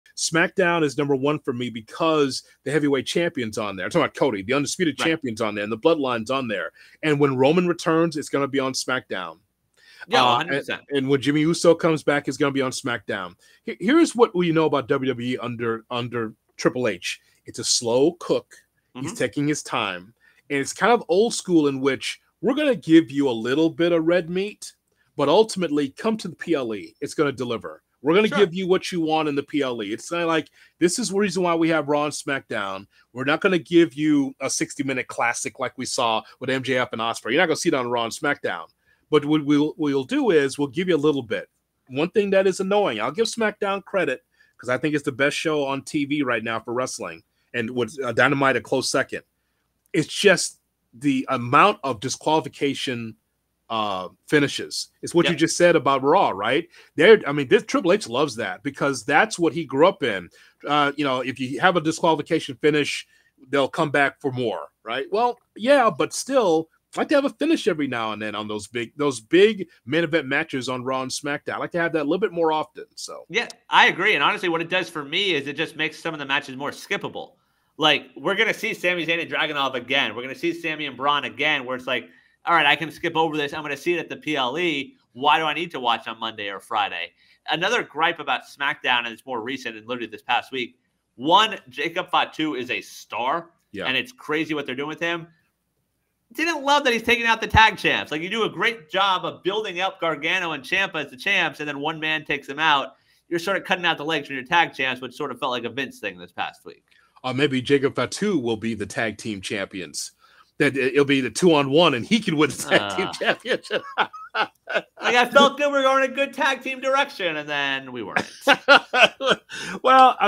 HHH Booking is a Slow Burn
GKW says that the booking of HHH takes some time but is worth it in the end.